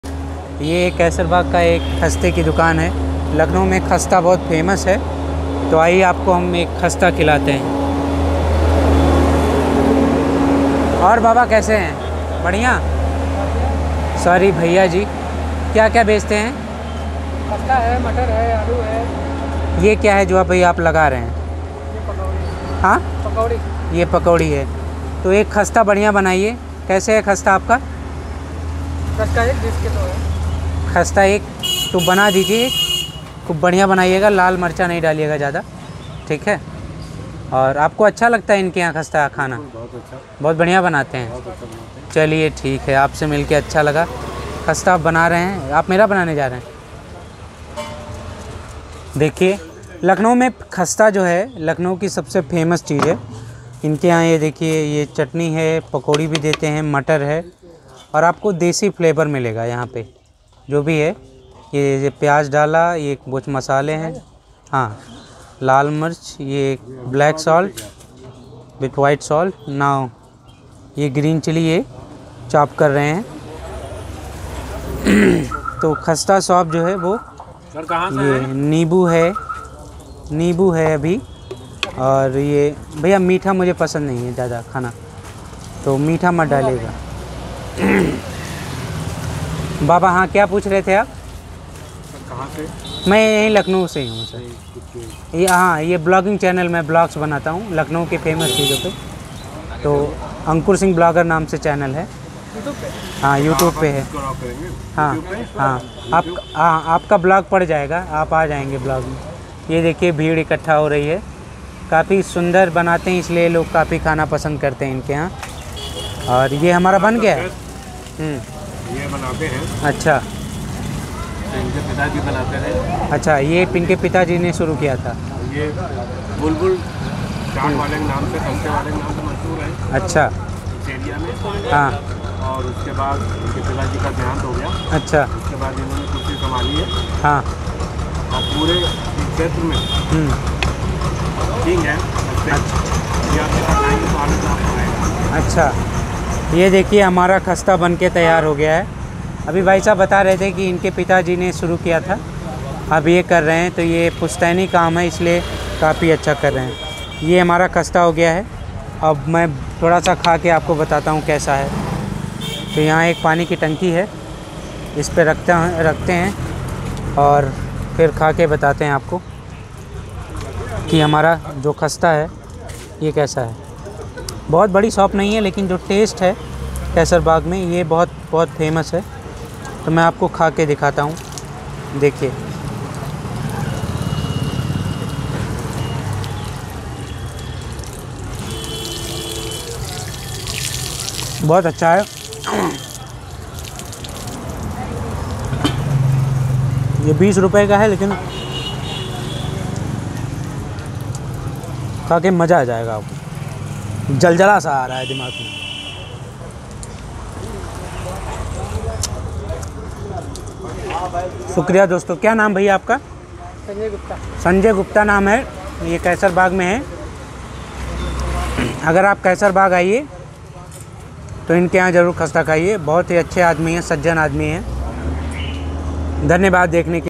Khasta chaat in Lucknow | Finger licking 😋 food
It's hard to translate the Hindi word "khasta," often associated with savory Indian snacks, unless you use a combination of terms like, "crispy, crackly, flaky, brittle." But even then, they come short.

If you really want to know what "khasta" tastes and feels and sounds like, you should try a kachori.

Kachoris were popularized around India by the Marwaris, traders from the western Indian state of Rajasthan, who settled across the length and breadth of the country, bringing with them delicious, if often rich and indulgent, foods.
 Thank-you so much for your reading.

for more videos of Lucknow
open the given link.